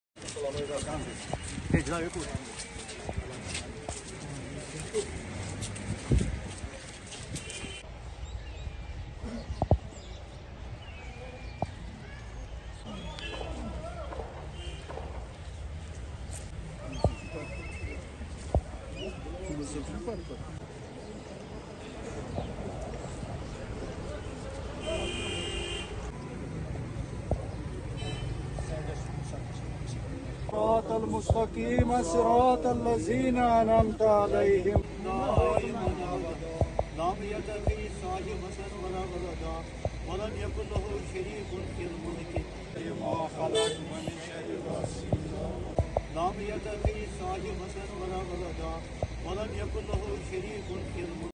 Dr. Farooq Abdullah and Omar Abdullah offered fatiha & floral tributes at the final resting place
Dr. Farooq Abdullah and Omar Abdullah offered fatiha & floral tributes at the final resting place of Sheikh Nazir Ahmad Sahib at Malik Sahib Soura Graveyard.